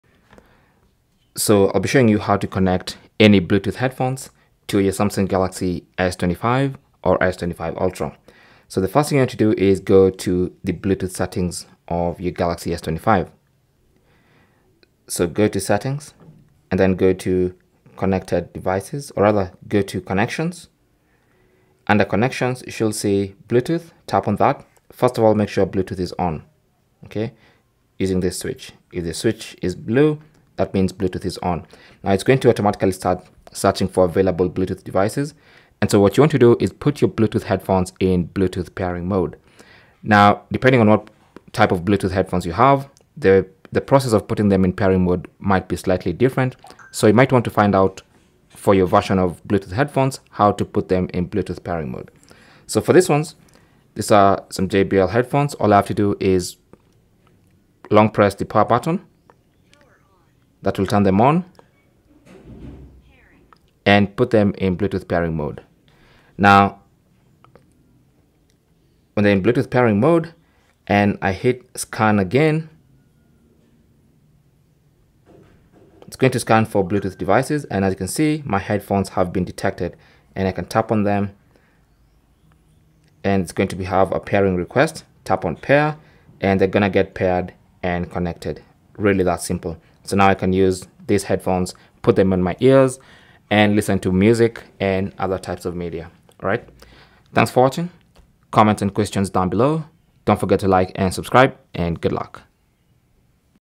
How To Connect Bluetooth Headphones To Samsung Galaxy S25/S25 Ultra
Learn how to connect Bluetooth headphones to Samsung Galaxy S25/S25 Ultra and enjoy wireless audio with ease.

Connecting Bluetooth headphones to Samsung Galaxy S25/S25 Ultra lets you listen to music, take calls, and watch videos without the hassle of wires. With a few simple steps, you can pair your Bluetooth headphones to Samsung Galaxy S25/S25 Ultra and start using them right away. Whether you're setting up new headphones or reconnecting an existing pair, the process is quick and straightforward.

Open Settings and select 'Connections'.

Proceed and turn on Bluetooth.

Put your headphones in pairing mode.

Scan for devices and select your headphones.

Tap Pair to connect.

Cell Phone Tripod Adapter